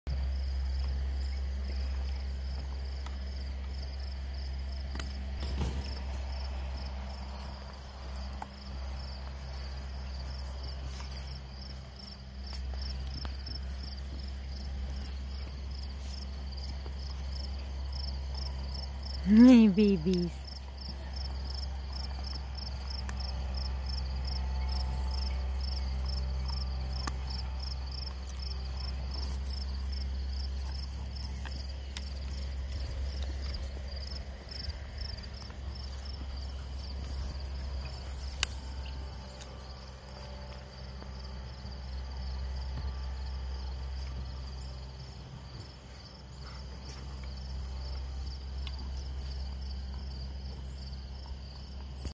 Appa and Naga love sharing
my Great Pyrenees Appa (15 month old male) & Naga (6mo old female) playing.
Also Coco and Mocha the goats :) in the background